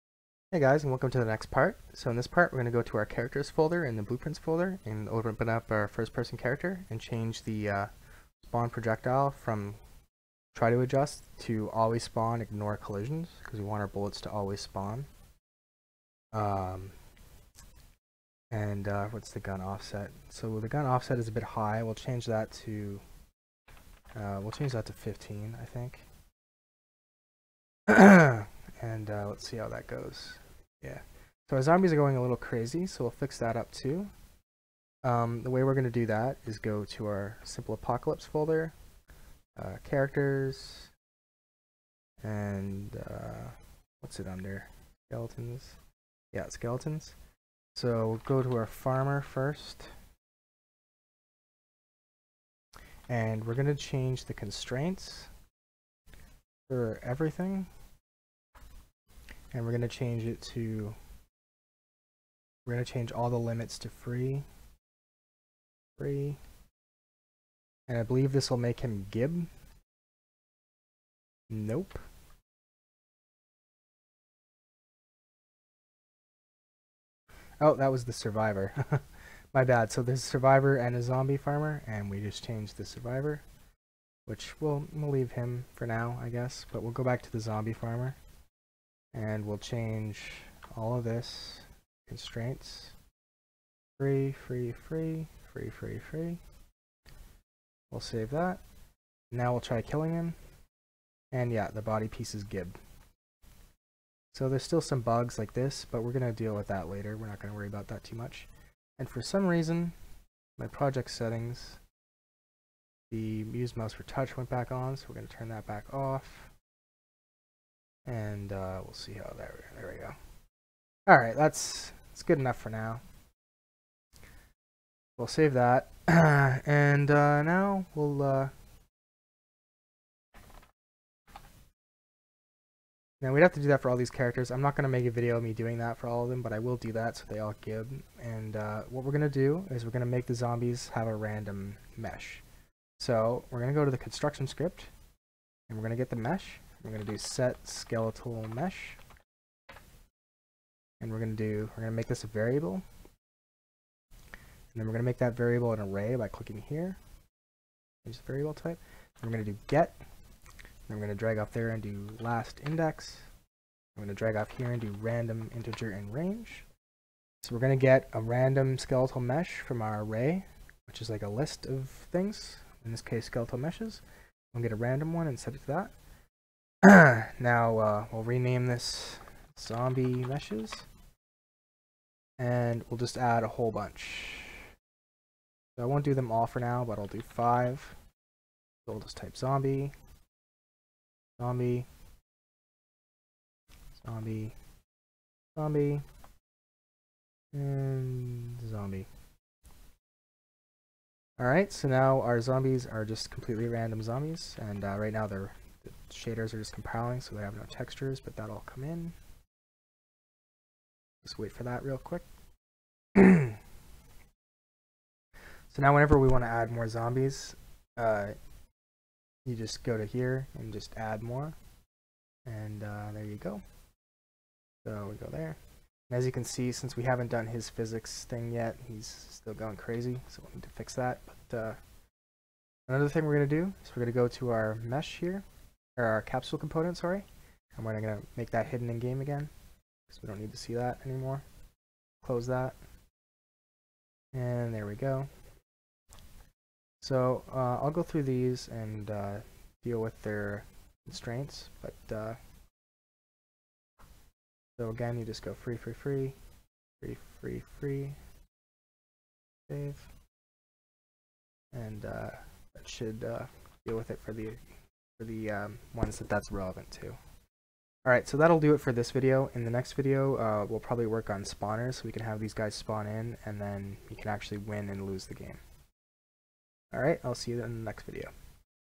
Creating a VR Zombie Game With UE4 Blueprints (Part 3 - Minor Updates)
In this episode we randomize the meshes of zombies, fix bullet spawning, and get rid of the capsule visibility

All Assets from the Unreal Marketplace

Simple Apocalypse by Synty Studios
Simply Apocalypse Interiors by Synty Studios
Zombie Starter - MoCap Pack by Motion Capture Online